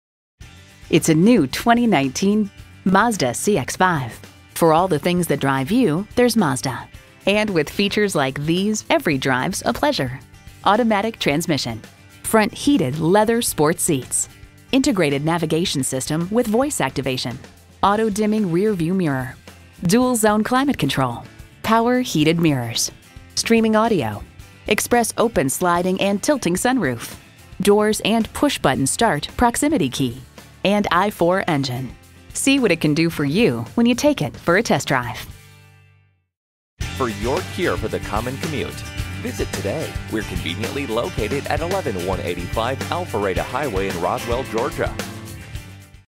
New 2019 Mazda CX-5 Roswell GA Atlanta, GA #654997 - SOLD
Year: 2019
Make: Mazda
Model: CX-5
Trim: Grand Touring
Engine: 2.5L 4-Cylinder
Transmission: 6-Speed Automatic
Color: Blue
Interior: Parchment
Stock #: 654997
VIN: JM3KFADM7K0654997
Price includes: $1,000 - Customer Cash. Exp. 09/03/2019 Recent Arrival! New Price! $2,464 off MSRP! Priced below KBB Fair Purchase Price! Deep Crystal Blue 2019 Mazda CX-5 Grand Touring FWD 6-Speed Automatic 2.5L 4-CylinderbrWindow Tint $299 included.br25/31 City/Highway MPG

Address:
11185 Alpharetta Hwy